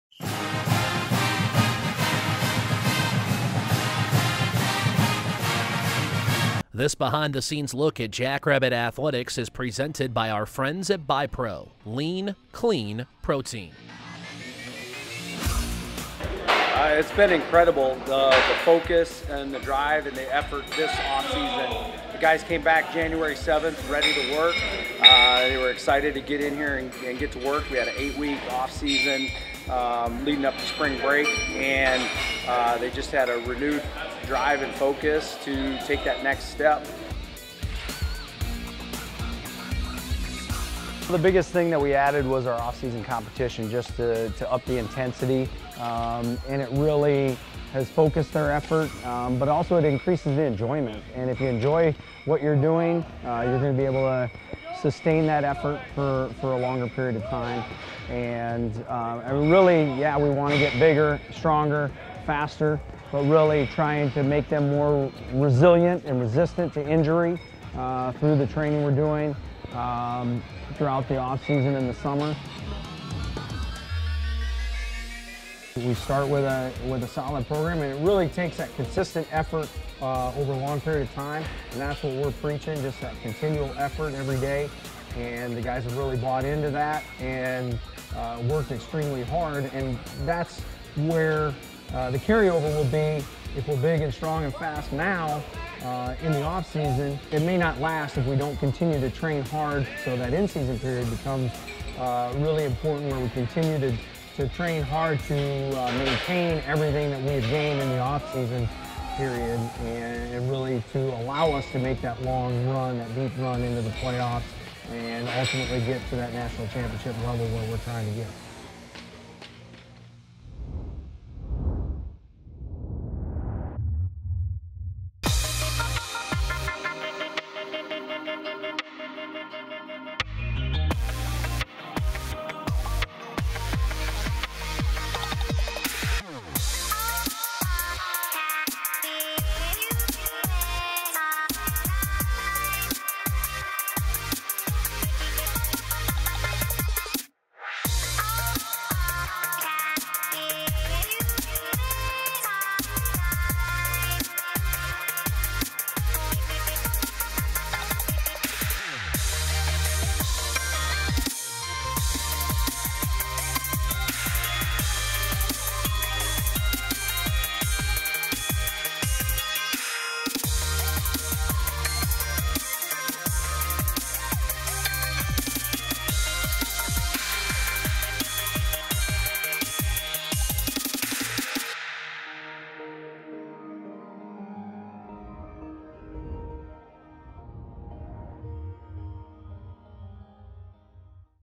Jackrabbit Athletics BiPro Series - Football Offseason Improvement
This video presented by BiPro provides a behind the scenes look at the offseason training of the South Dakota State Football team.